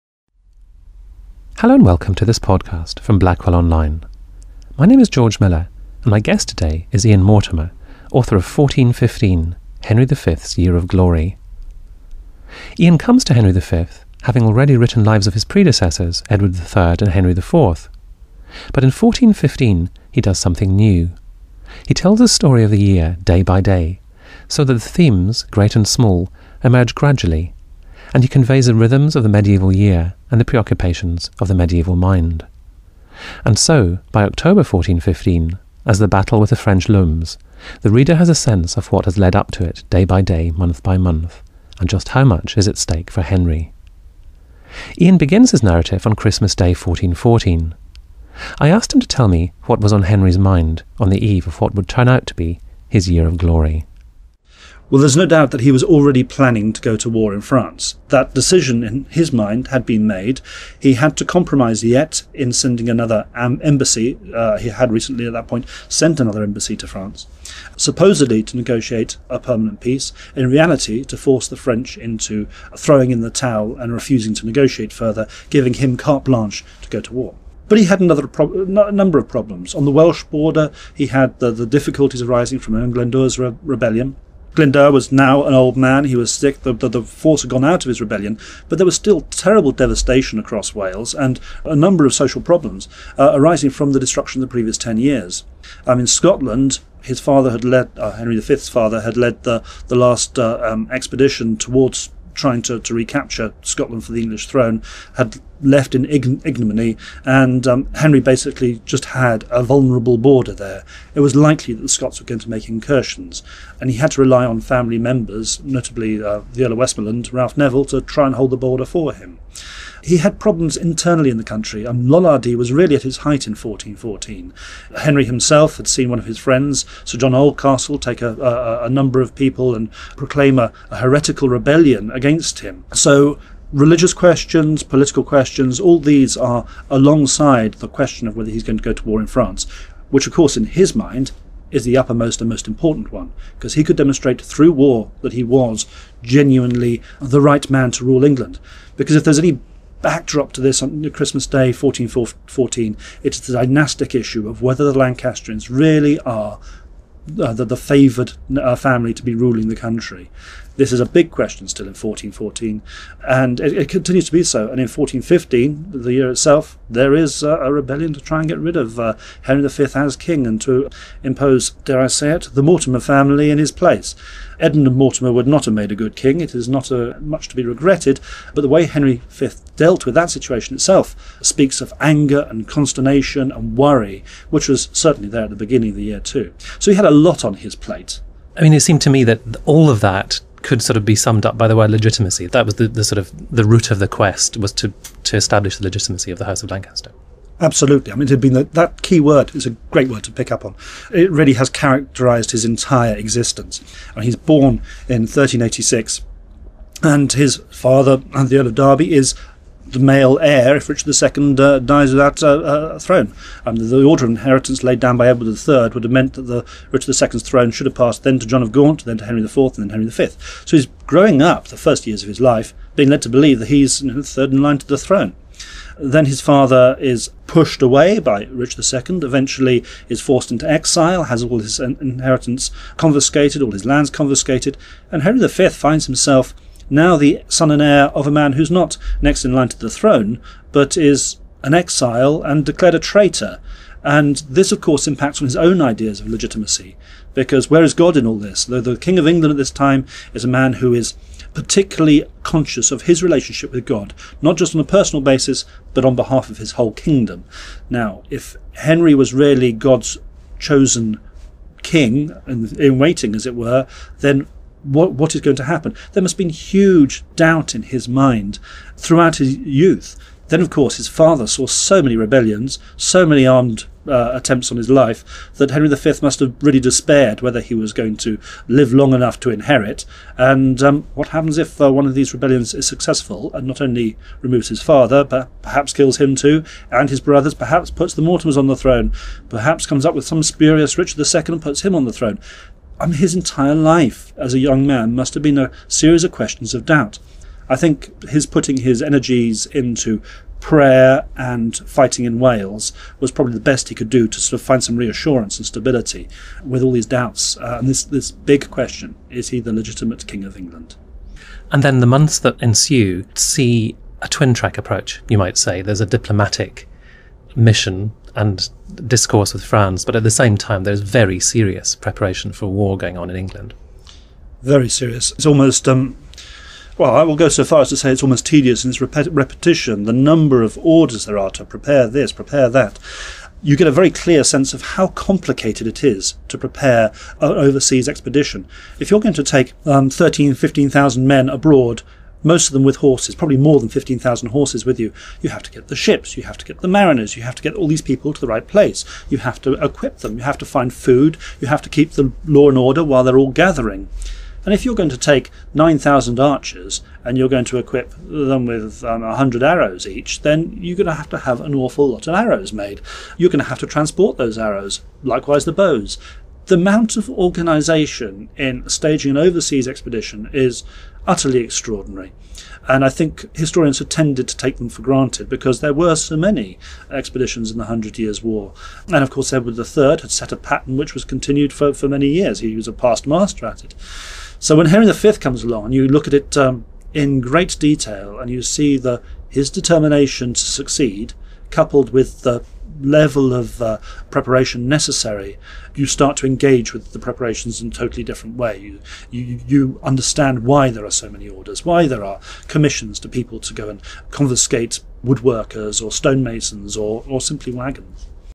Ian Mortimer - 1415 - Part 1 of 2
In his groundbreaking new book '1415', Ian Mortimer looks at Henry V's 'year of glory', a period of religious persecution, personal suffering and the Battle of Agincourt, and asks whether the King deserves to be remembered as the greatest man who ever ruled England.